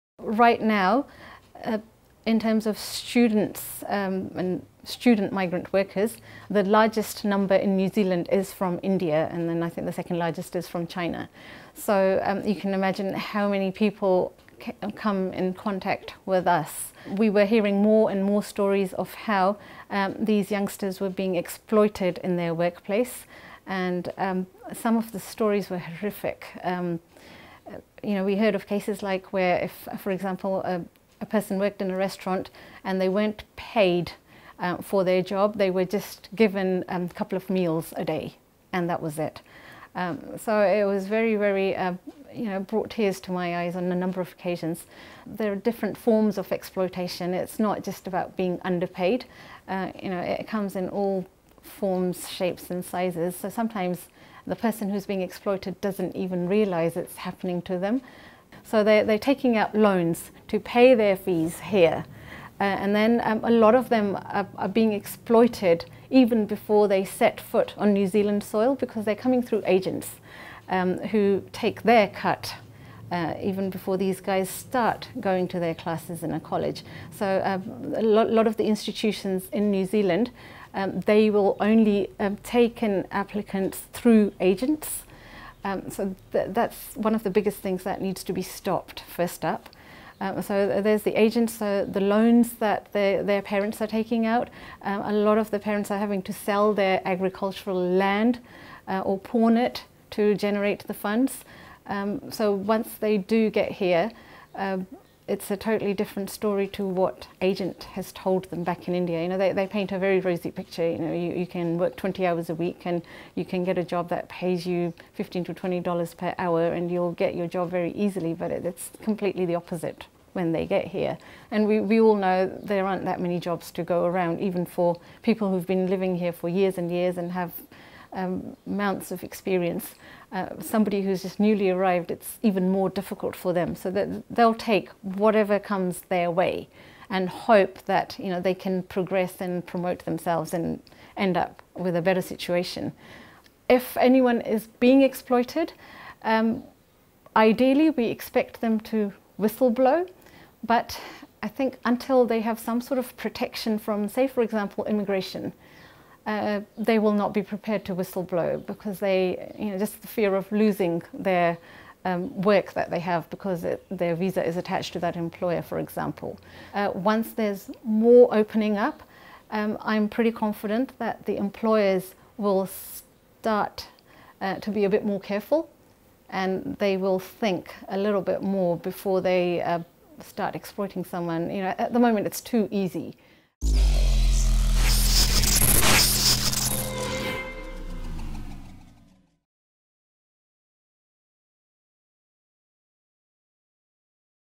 Young migrants speak out about workplace abuse: Anu Kaloti
Migrant Workers Association says the exploitation of international students often starts back at home.

Video shot & edited by Luke McPake